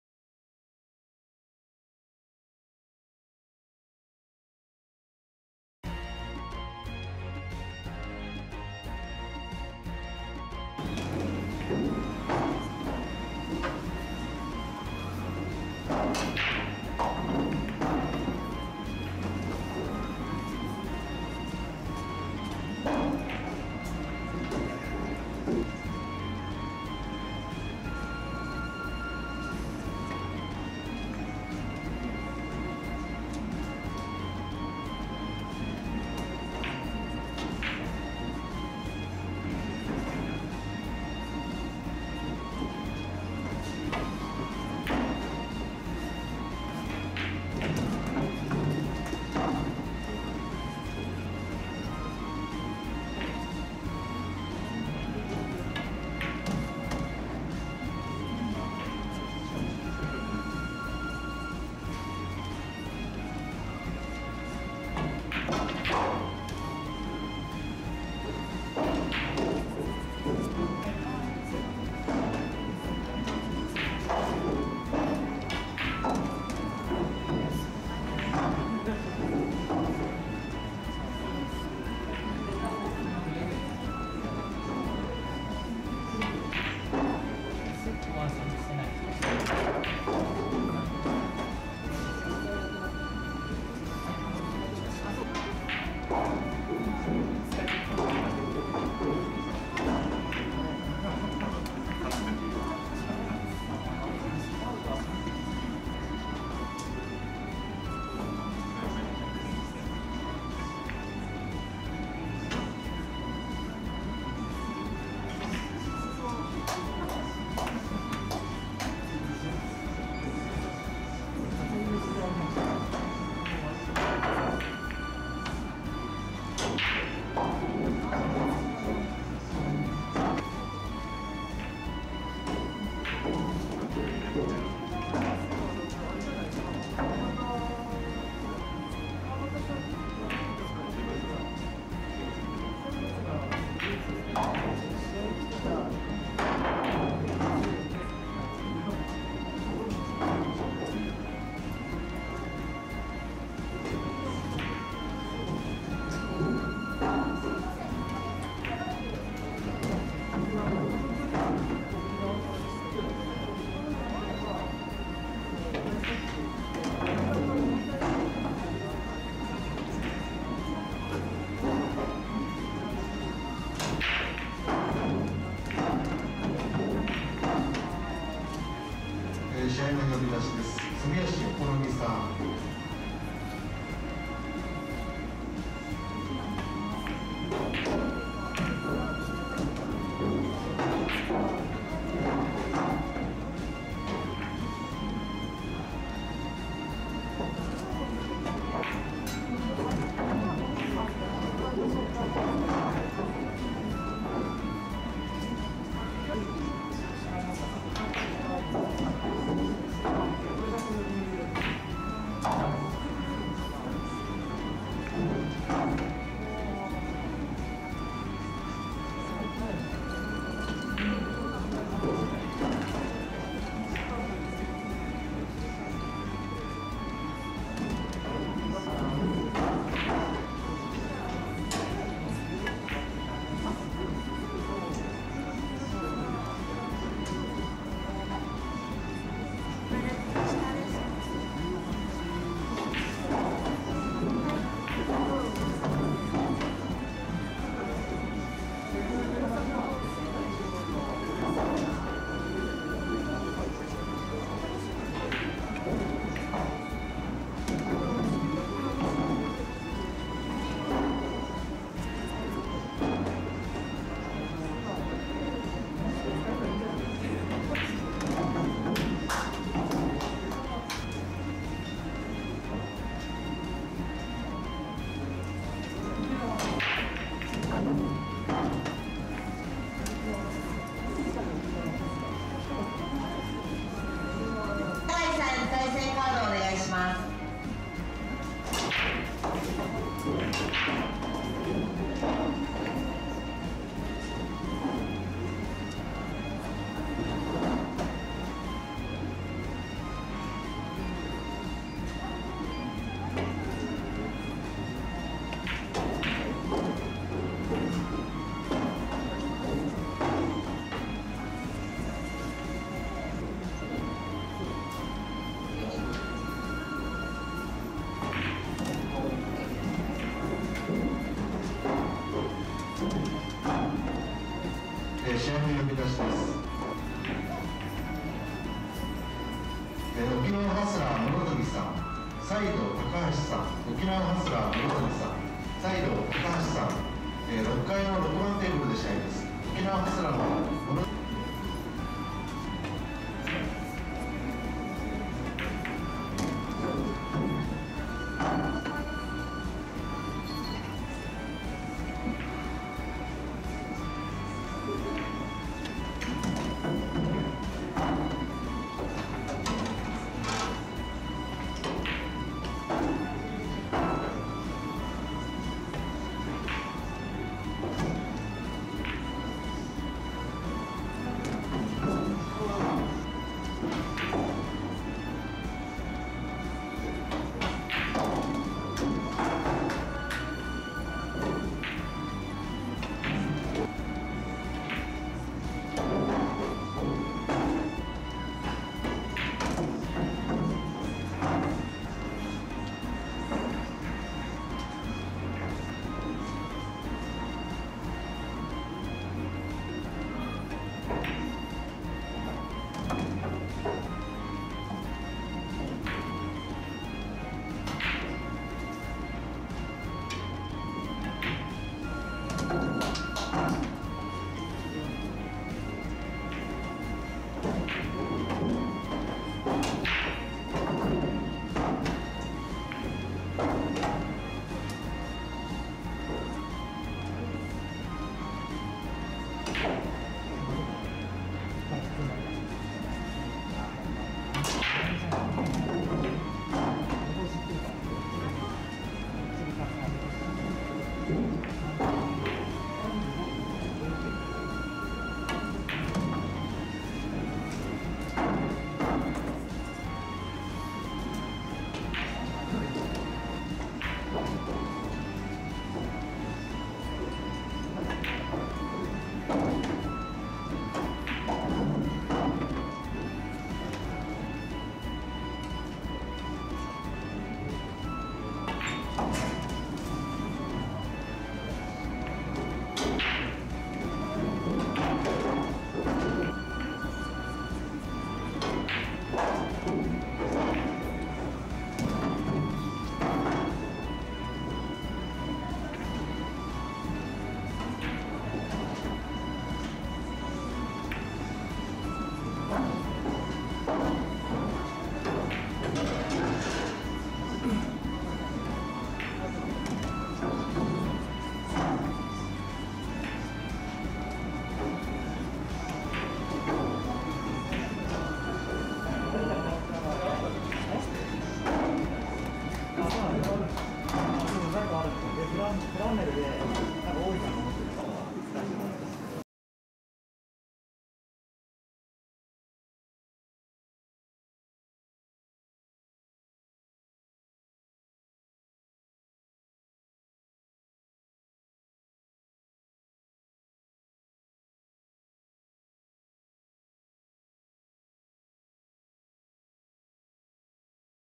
大屋亜梨須VS増田薫2020年12月13日ナインボールクラシックレディース予選（ビリヤード試合）
2020年12月13日東京都渋谷区CUEで行われたナインボールクラシックの試合動画です
フランネルの大屋さんとBreakの増田さんの対戦です

プールカイザー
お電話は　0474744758
チャンネルURL